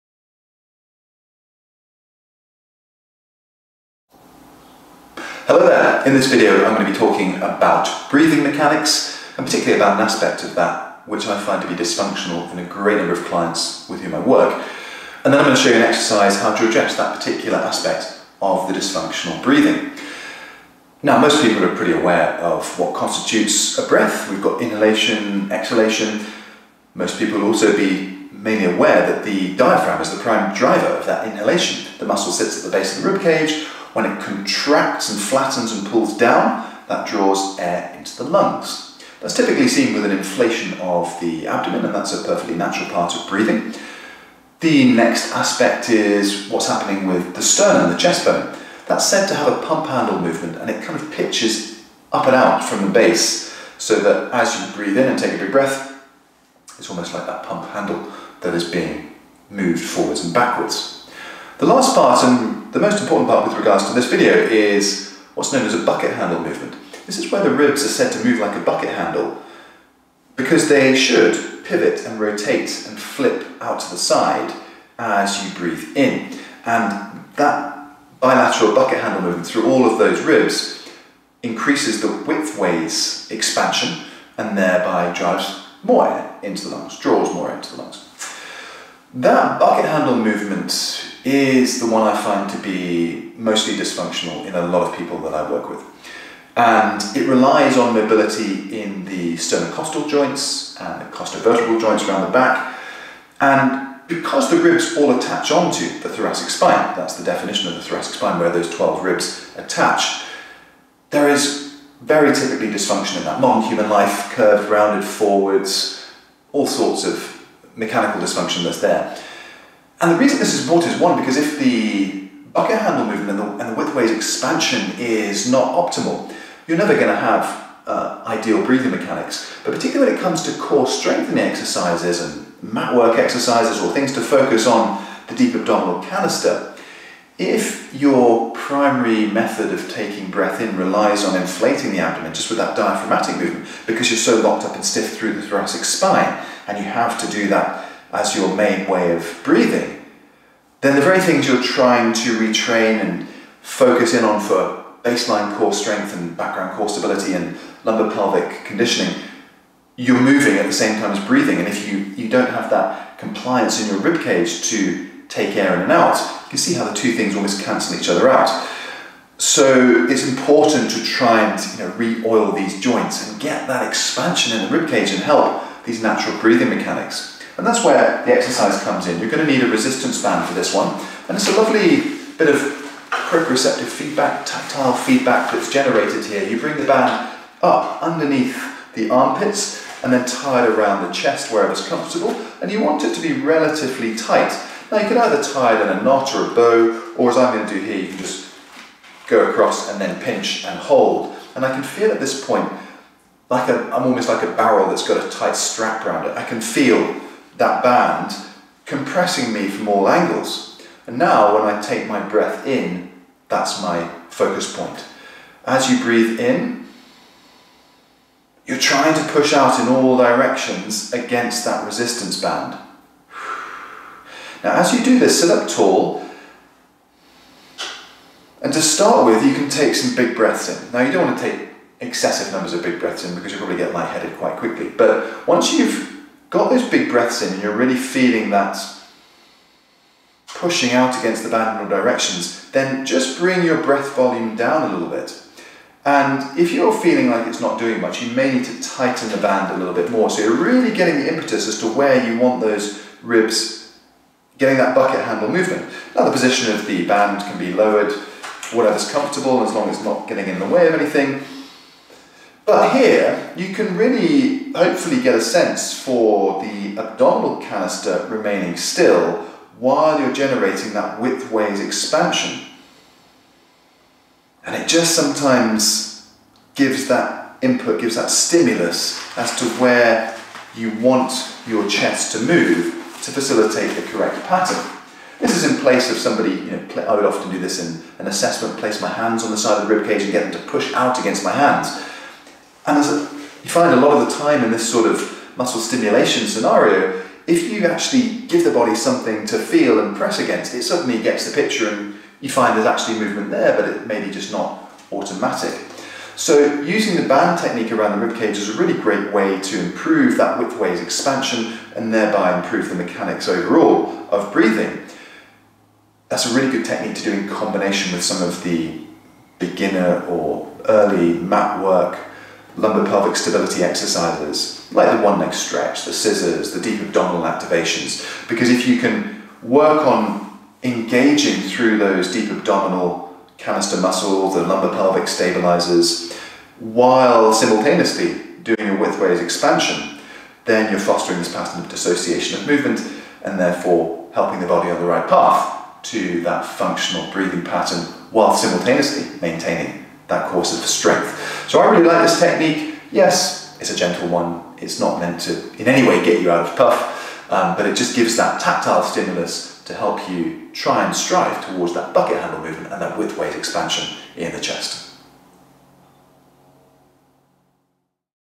Improving Breathing Mechanics - increasing chest expansion
One of the common areas of breathing mechanics dysfunction is poor movement of the chest wall. This is typically linked to broader thoracic dysfunction, and without the necessary expansion in the ribcage, others drivers of inhalation tend to do the work instead.

This video discusses how to improve that with the aid of a resistance band exercise.